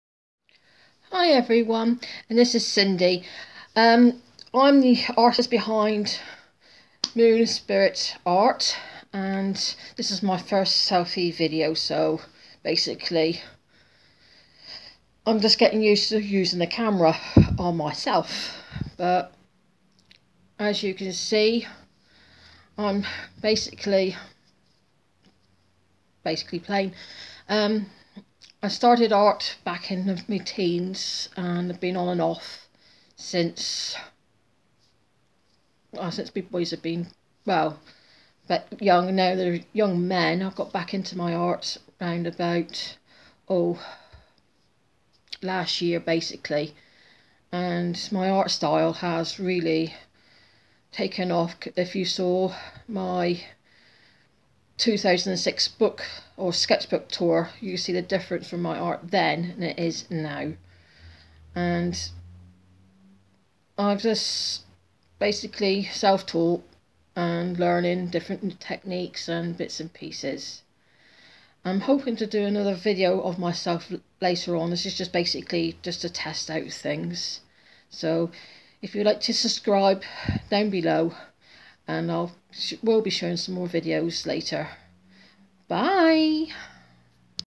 Me behind Moonspiritart.
This is just an test video .as you can see I am an little nervous. Hopefully I will get better

Samsung J3 mobile phone
Video edited with video editor for YouTube